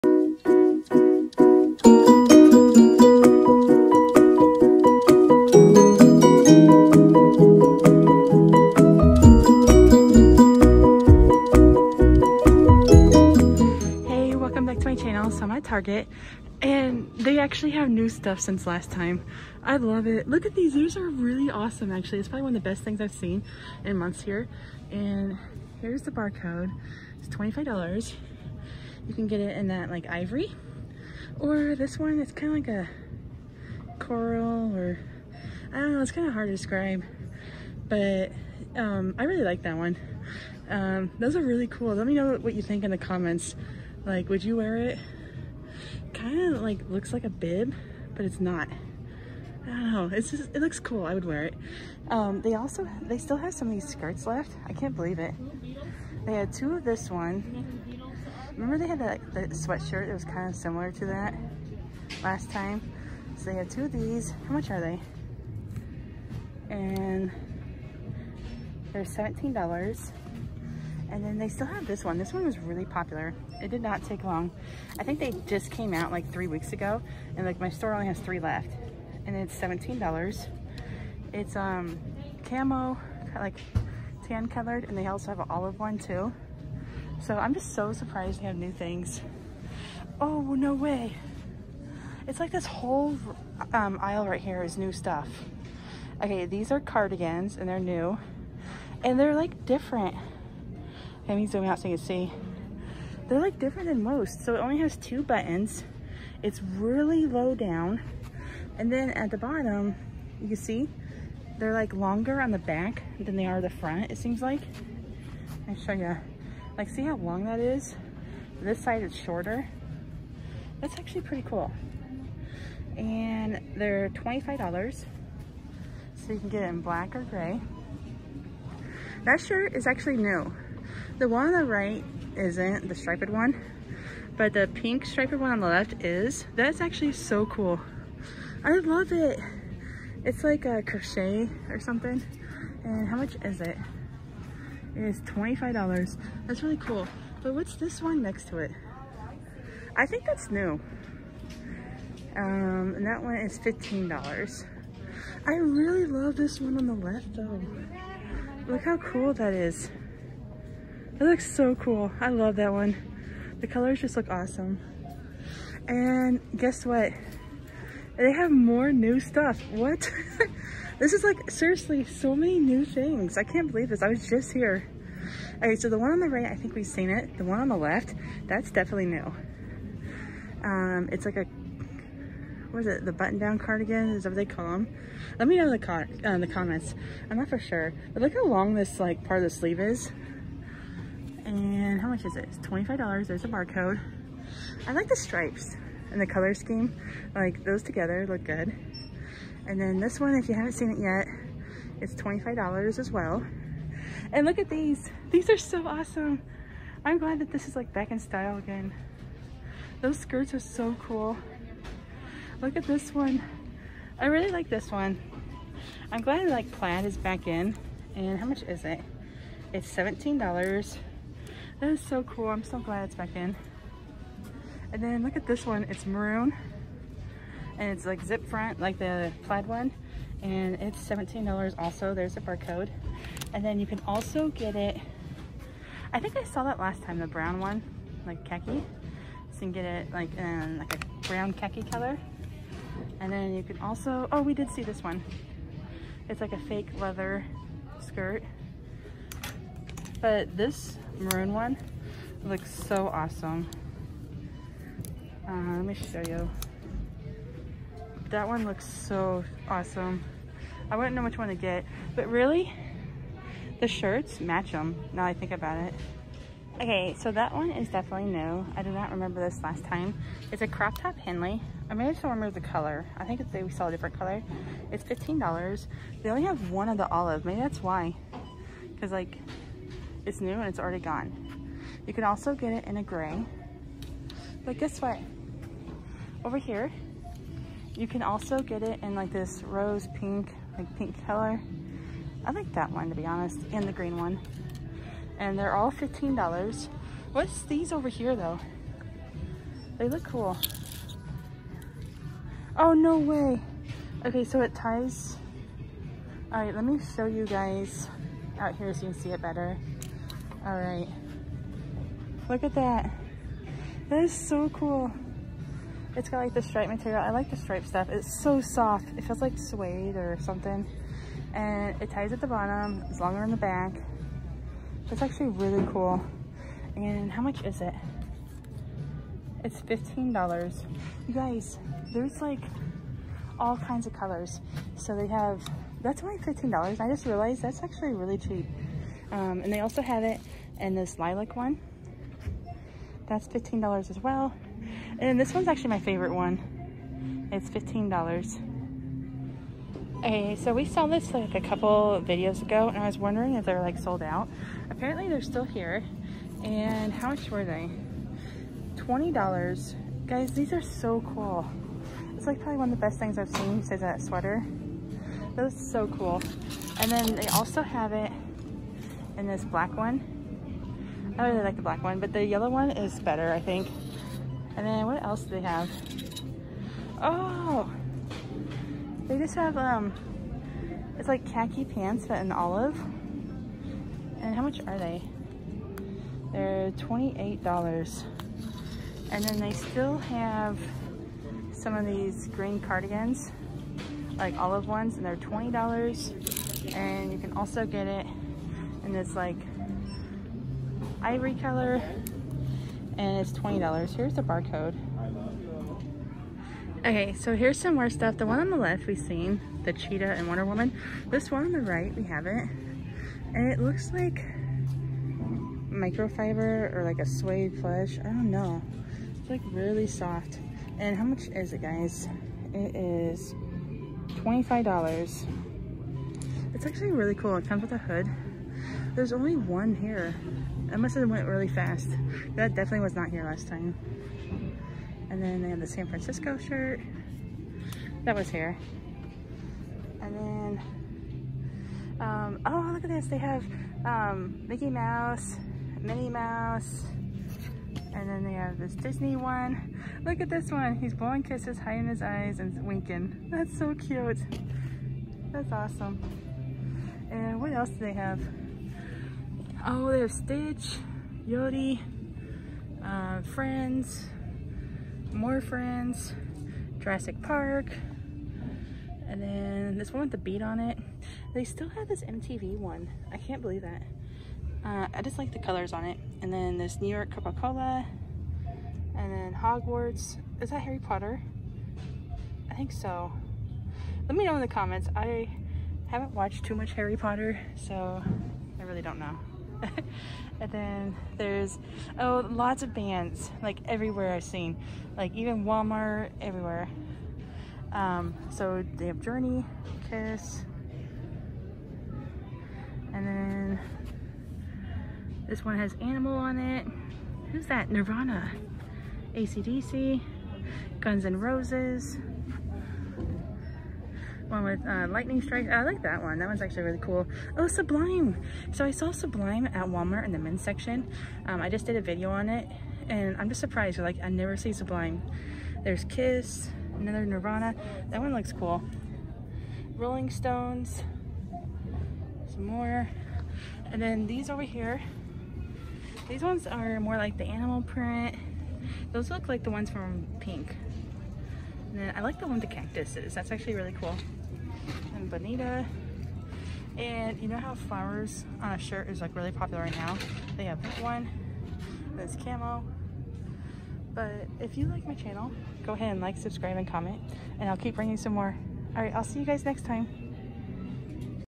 TONS OF NEW TARGET CLOTHING! TARGET CLOTHING HAUL! TARGET FALL CLOTHING! TARGET SHOP WITH ME!
Come with me to Target as I found tons of new finds! So many trending items and Fall clothing!